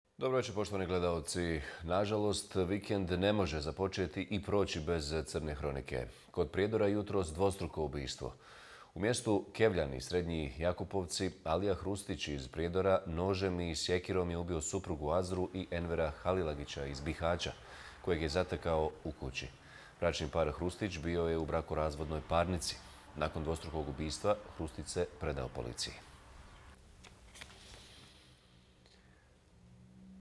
Dvostruko ubistvo kod Prijedora
Ubistvo, u mjestu Kevljani – Srednji Jakupovci, kod Prijedora, potvrđeno je Feni iz Okružnog javnog tužilaštva Banja Luka.

Alija je ubio Azru Hrustić (1982) i Envera Halilagića (1976) iz Bihaća.

Nezvanično, bračni par Hrustić su bili u brakorazvojnoj parnici i nisu proteklih mjeseci živjeli zajedno, a Alija je Halilagića zatekao u kući.

Alija Hrustić se nakon dvostrukog ubistva predao policiji.

Iz Okružnog javnog tužilaštva Banja Luka potvrdili su Feni da je uviđaj obavio dežurni tužilac Siniša Marković, zajedno s pripadnicima Policijske uprave Prijedor.

Tužilac je naložio da se preduzmu istražne mjere i radnje, kao i vještačenja radi utvrđivanja okolnosti počinjenog krivičnog djela.

Naložena je o obdukcija tijela ubijenih, koja će biti obavljena u Zavodu za sudsku medicinu.

Nakon završene kriminalističke obrade, Alija Hrustić će, uz izvještaj o počinjenom krivičnom djelu teškog ubistva, biti sproveden u Okružno javno tužilaštvo Banja Luka radi ispitivanja.

Istraga je u toku.